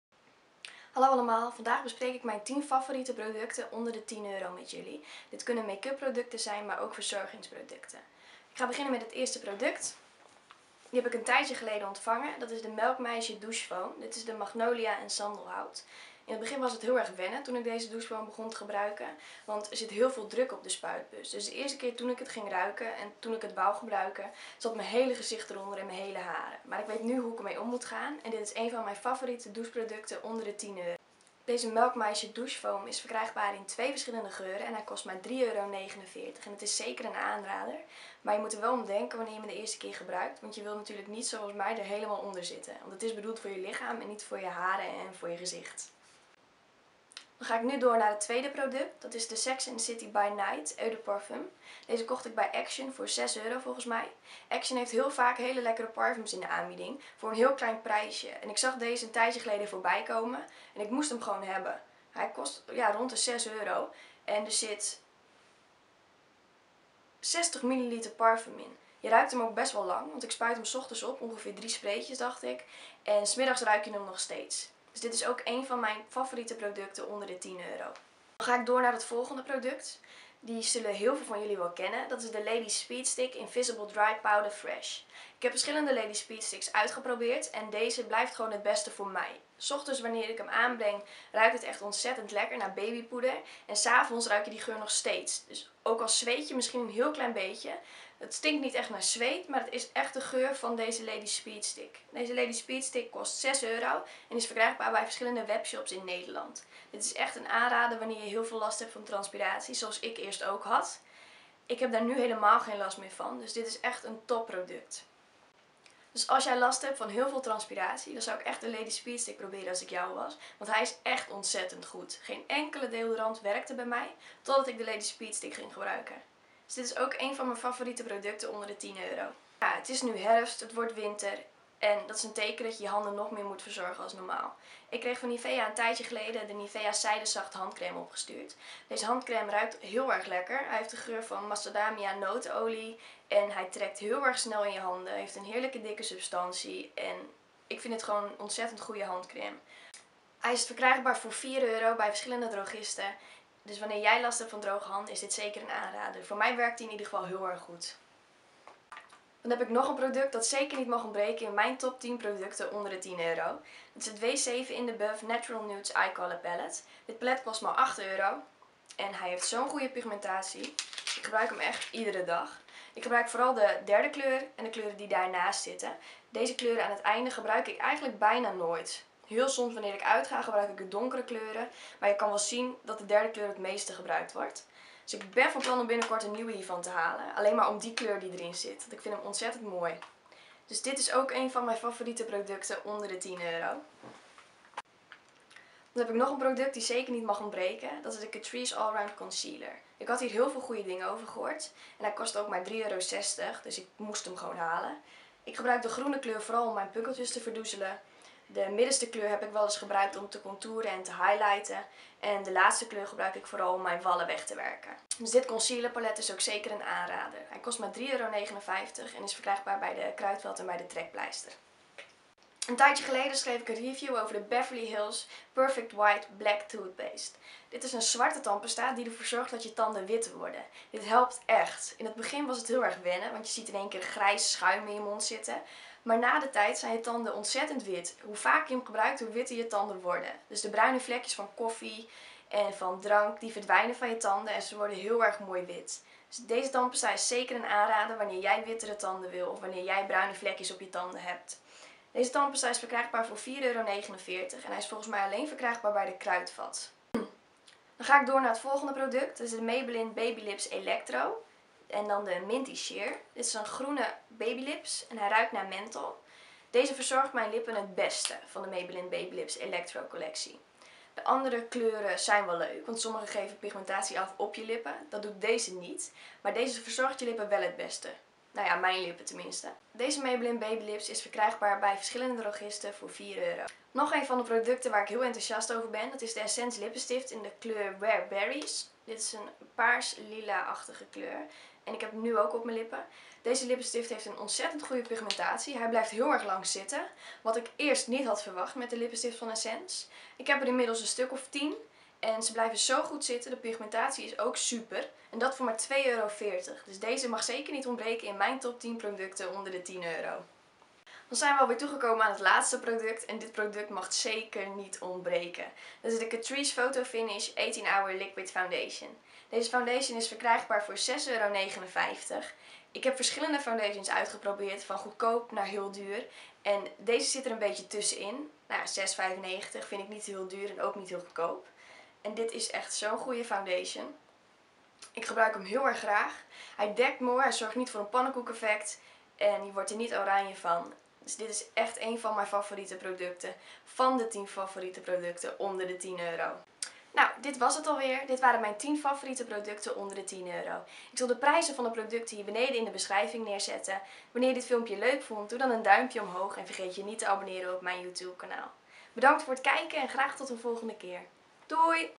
Top 10 Producten onder de €10
Vandaag een video met mijn top 10 Beauty/Verzorgingsproducten onder de  €10,00.

*De producten die ik bespreek in de video:

- Melkmeisje Douche Foam ( € 3,49 )
- Sex and The City By Night ( € 6,00 ? )
- Lady Speed Stick Powder Fresh Invisible Dry ( € 6,00 )
- Nivea Zijdezacht Handcrème ( € 4,00 )
- W7 In The Buff  ( € 8,00 )
- Catrice Alround Concealer ( € 3,59 )
- Beverly Hills Tandpasta ( € 4,49 )
- Maybelline Baby Lips Electro ( € 3,99 )
- Essence Lipstick Wear Berries ( € 2,39 )
- Catrice Photo Finish 18h Foundation ( € 5,69 )

I n s t a g r a m : kristelovae